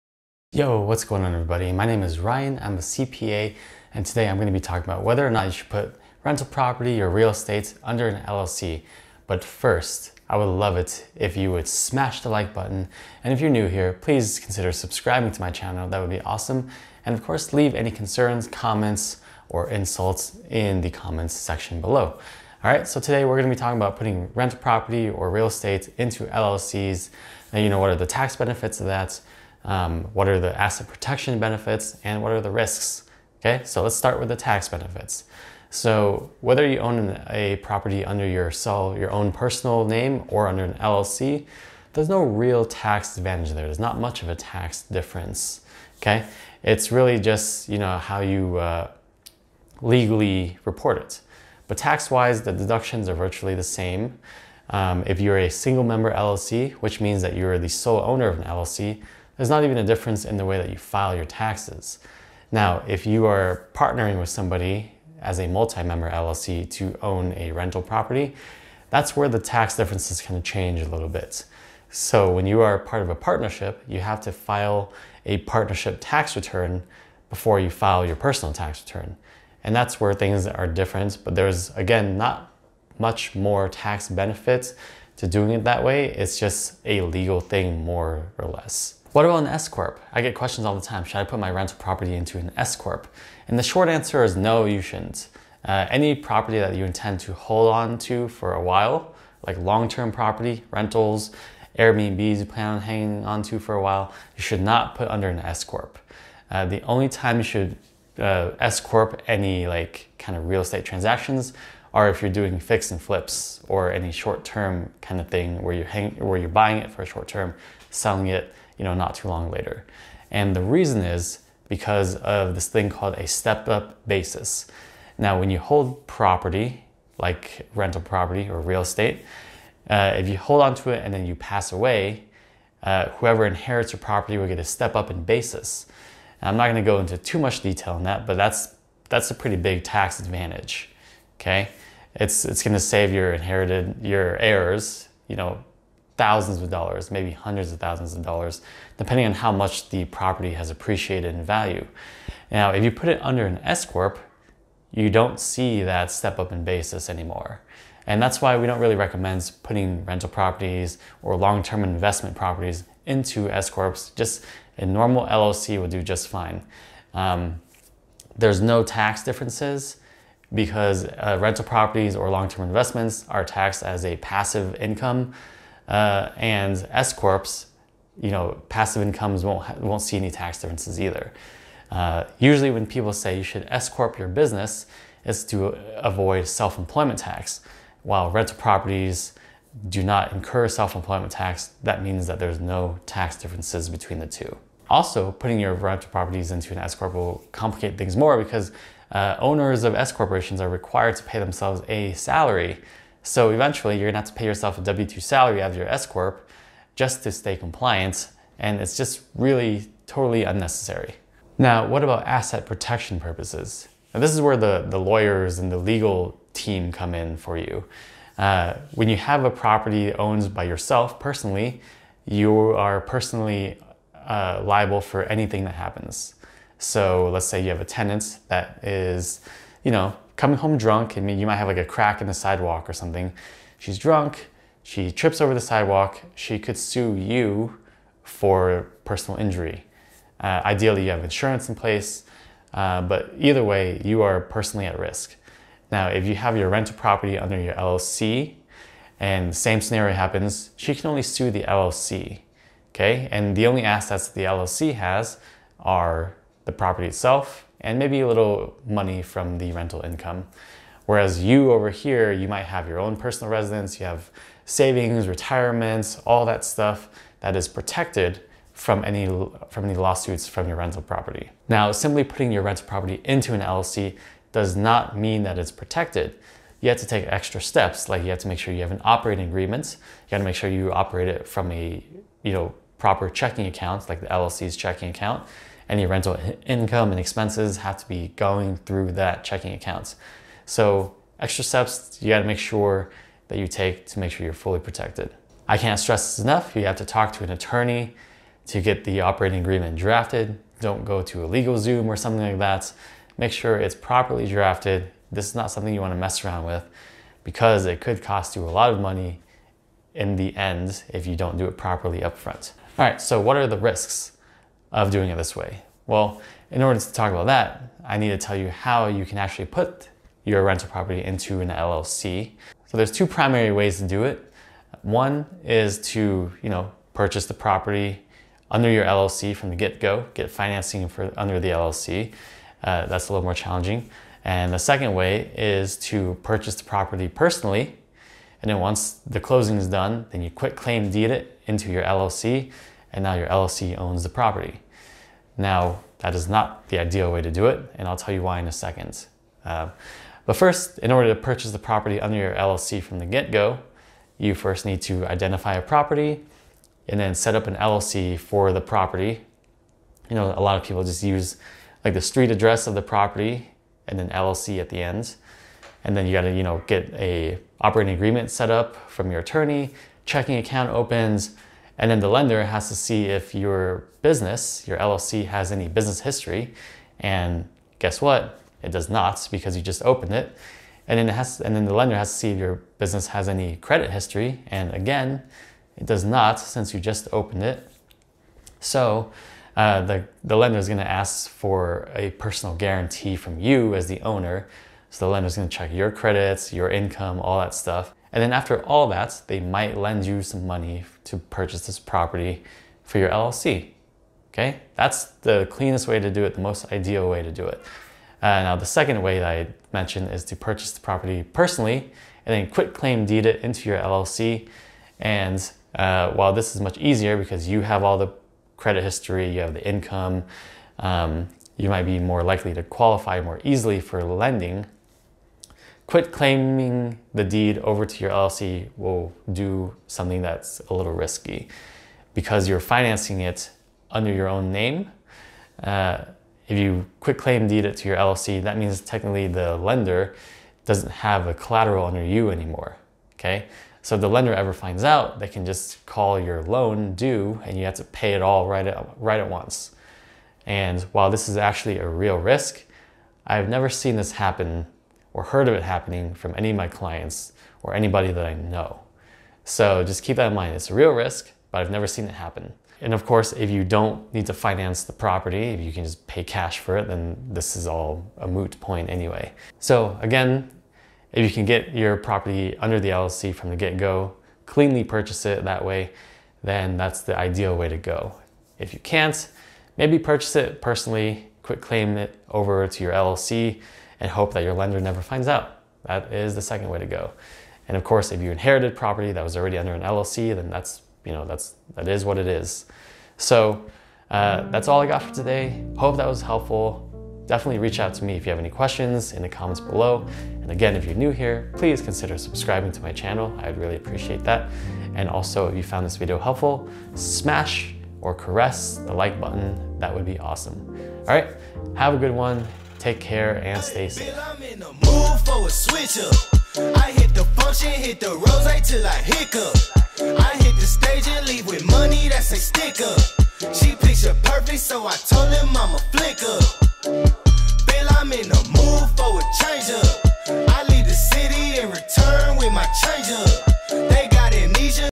Should You Put Rental Property In An LLC
In this video, I discuss the differences/benefits of putting your rental property in an LLC.  Should you put your rental property in an S Corp?  Maybe an LLC taxed as an S Corp?  Well, that's a great question!  You may have to watch the video to find out.